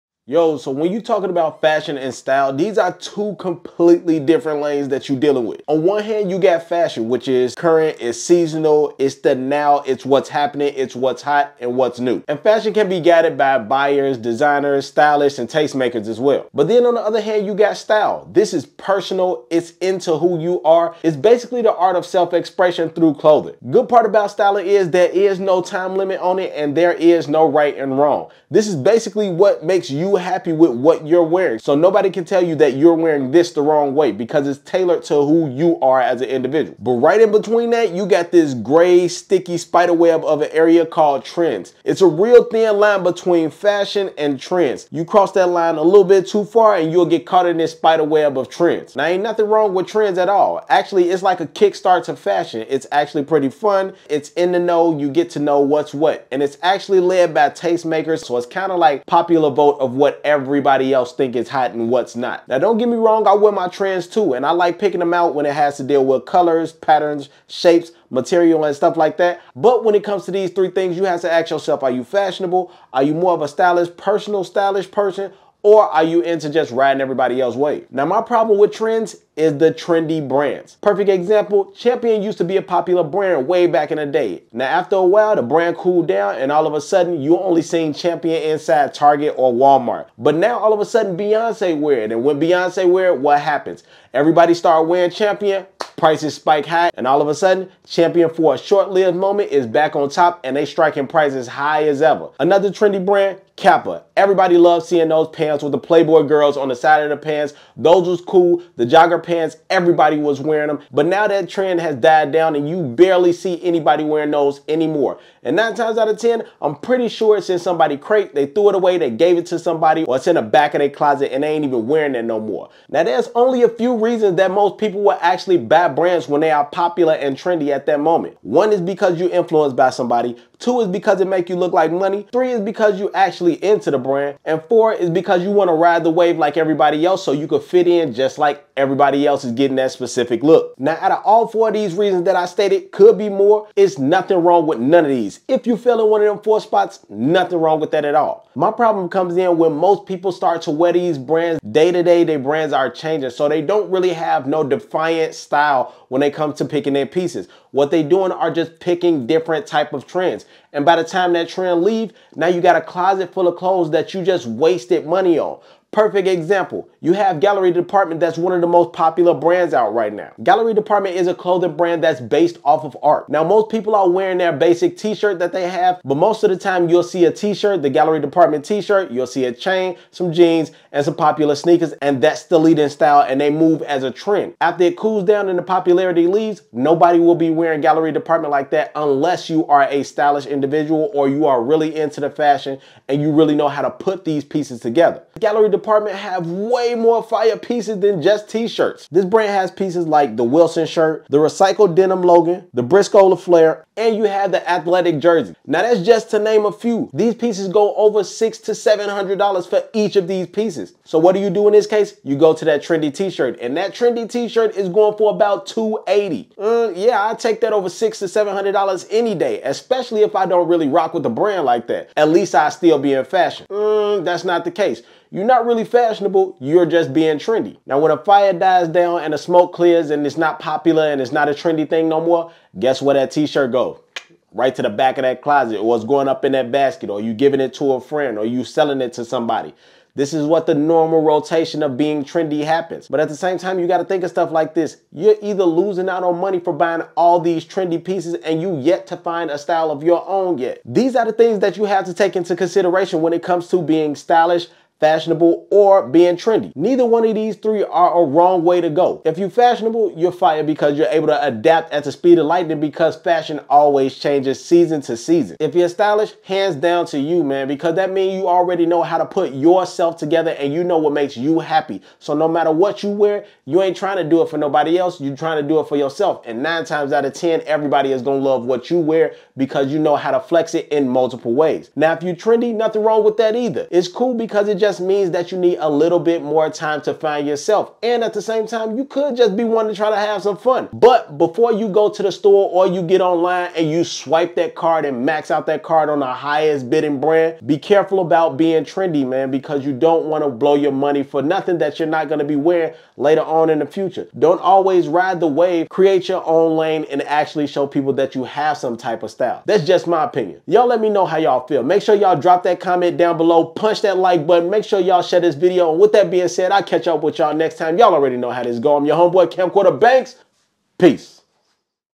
Don't Get Caught Being Trendy | Finding Yourself In Fashion | Men's Fashion 2024
It's not easy finding your style. But, while trying different looks, do you know which area you fall in? Are you fashion forward, stylish, or just trendy. Each of these are different but they can be very similar. Here's a few things to thing about when it comes to choosing one of these category of looks.